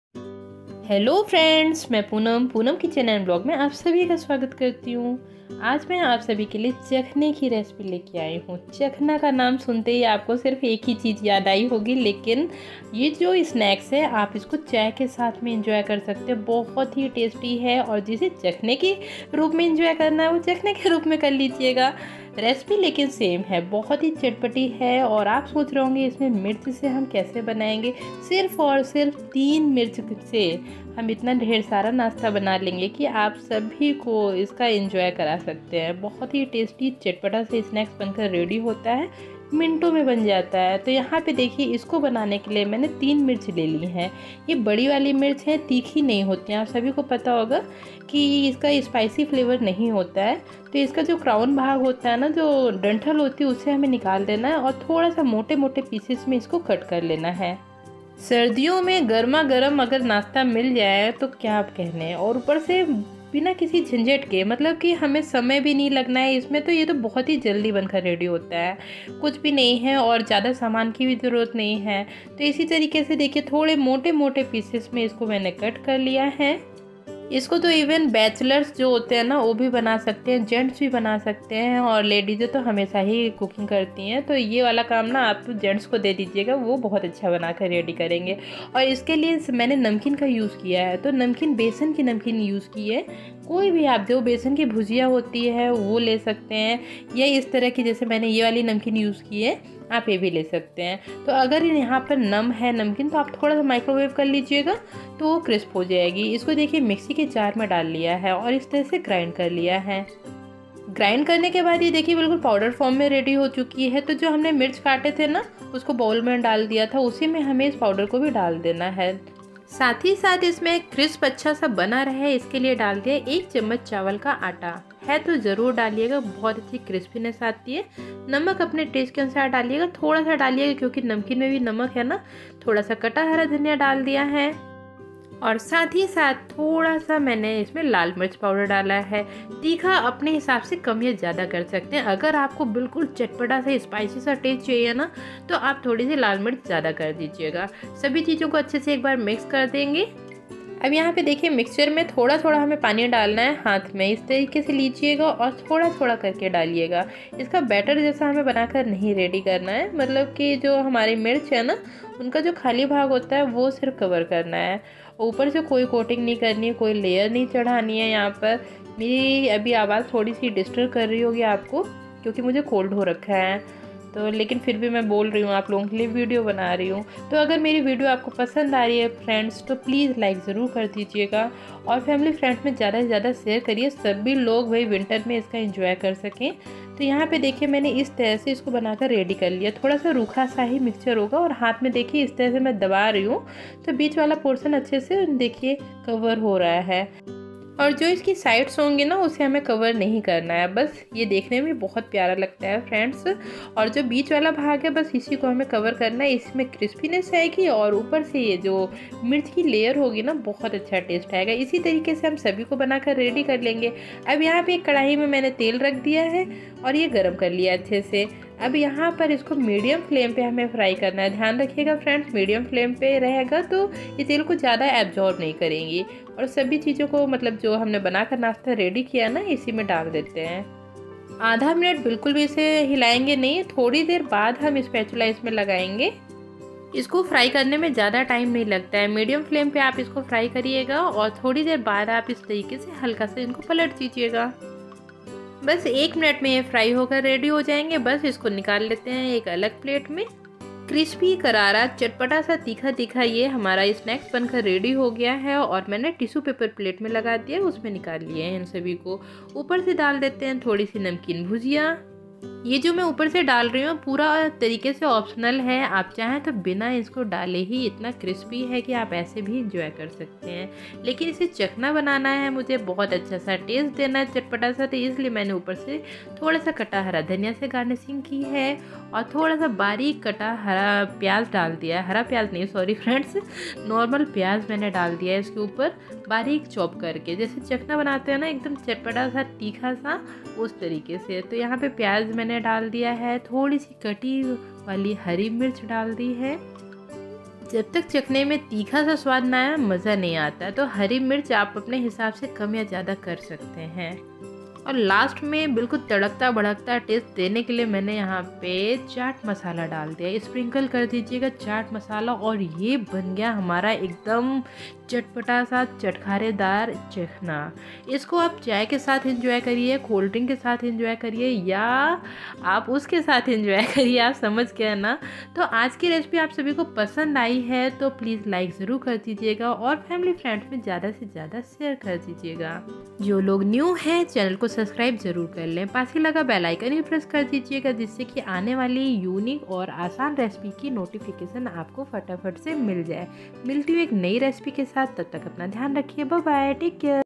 बाजार से लाने का झंझट ही खत्म अब घर पर सिर्फ 3 मिर्च से सस्ते में पूरे परिवार के लिए एकदम नई रेसिपी
hell friends 👋
mai Poonam, aap sabhi ke liye unique mirch ki recipe lekar aayi hai, recipe pasand aaye to like share jaroor se karna, aur jo new hai wo subscribe jarur kar le 🙏